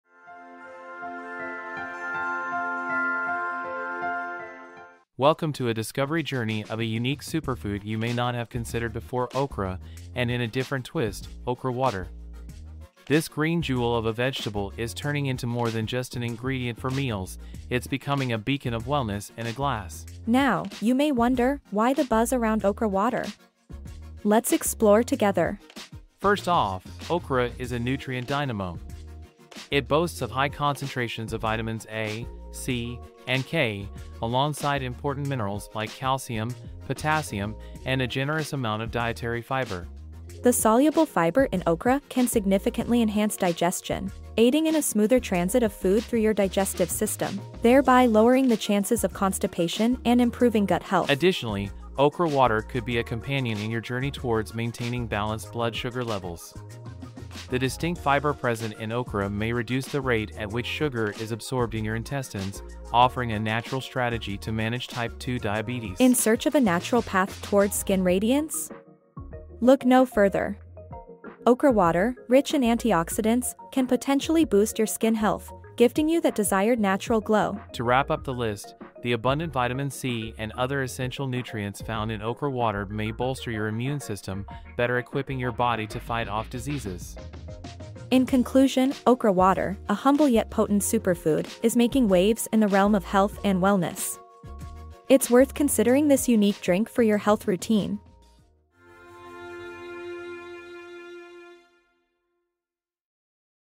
🍃Okra Water: Unveiling the Superfood Secret 💦
💧 Dive into the fascinating world of superfoods with our exploration of Okra Water 🌿! Bursting with nutrients and loaded with health benefits, this green elixir could be your next favorite wellness drink. From aiding digestion to balancing blood sugar levels, okra water is stirring up a storm in the wellness world 🌪️. Click to discover how this humble vegetable is becoming a beacon of health and well-being! OkraWater Superfoods HealthTrends"